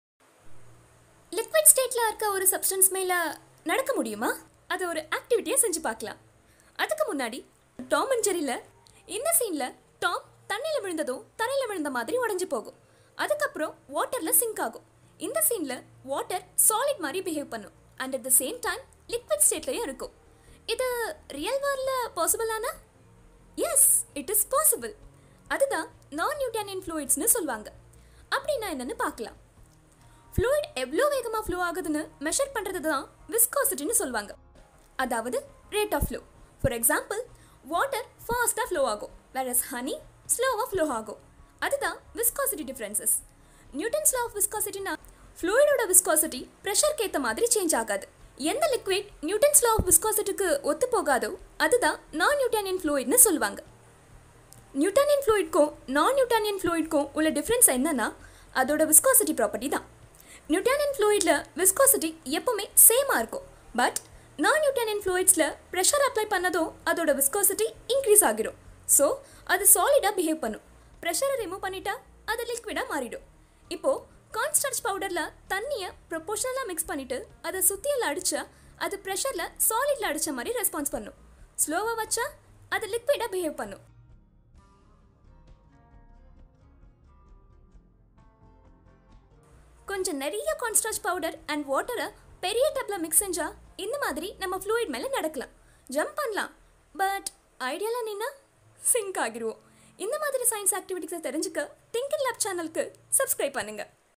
Walking on a fluid | Hands-on Science | Thinkin Lab
Yes, You can walk on a fluid by using this small science trick. These fluids are called Non-Newtonian fluids.

With the use of household materials here's how we're going to make the substance that is in a solid and a liquid state as well.

Any interesting topic, please comment we'll get into the insights.